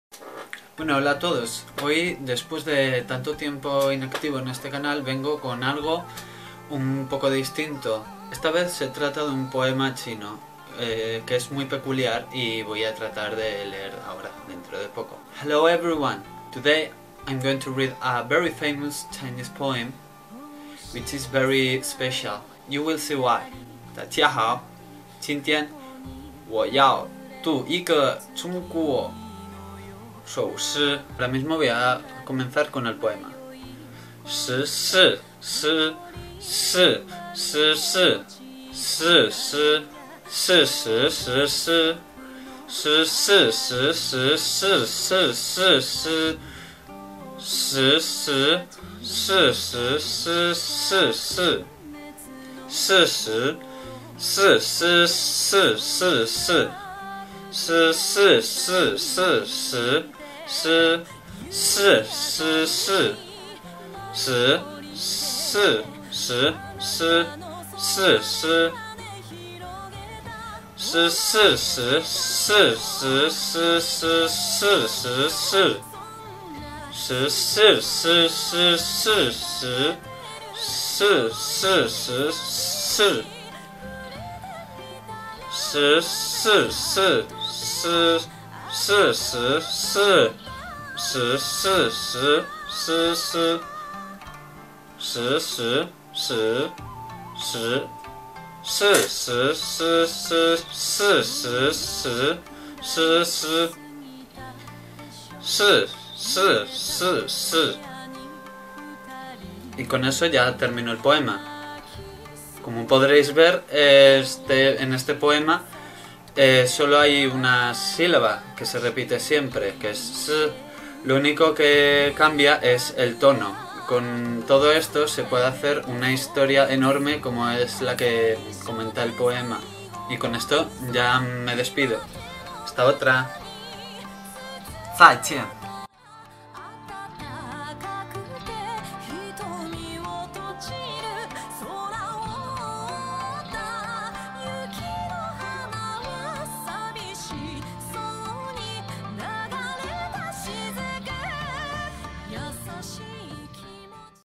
Chinese poem Challenge
Are you curious? Have you ever heard a poem in chinese? Check this! It's veeeery special! :D

This is the text of the poem in Mandarin, pinyin transcription and english.